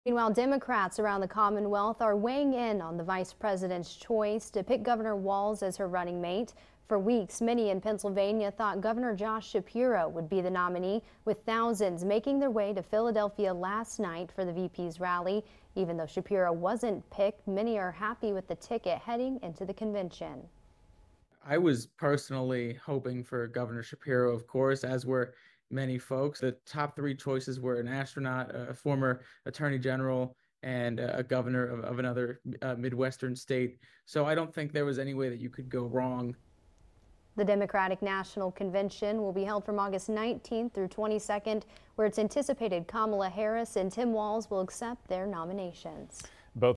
Democrats React to Walz Being Picked as V.P. Candidate
Democrats around the Commonwealth are weighing in on the Vice President's choice to pick Governor Walz as her running mate.

For weeks, many in Pennsylvania thought Governor Josh Shapiro would be the nominee, with thousands making their way to Philadelphia Tuesday night for the V.P.'s rally.

Even though Shapiro wasn't picked, many are happy with the ticket heading into the convention.